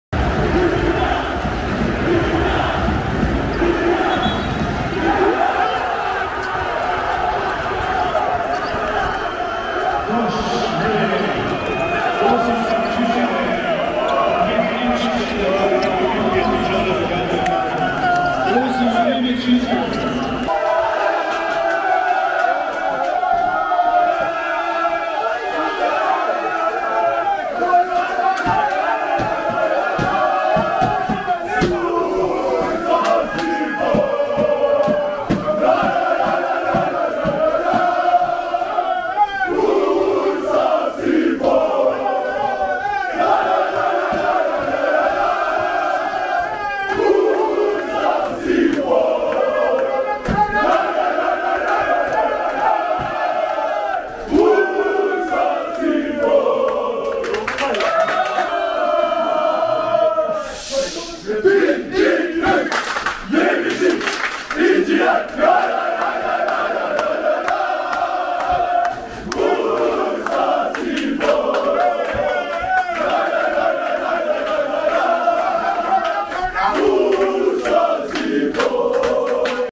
İşte Frutti Extra Bursaspor'a EuroCup'ta yarı final böyle geldi
İşte maçın sonunda yaşanan tarihi anlar ve Frutti Extra Bursaspor soyunma odası sevinci... 7Days Euro Cup B Grubu’nda play-off’a kalan ve Son 16 Turu’nda Sırbistan deplasmanında Partizan’ı uzatmada 103-95’lik skorla eleyip tarih yazan Frutti Extra Bursaspor, Slovenya’da Cedevita Olimpija takımı 85-83 yererek yarı finale yükseldi.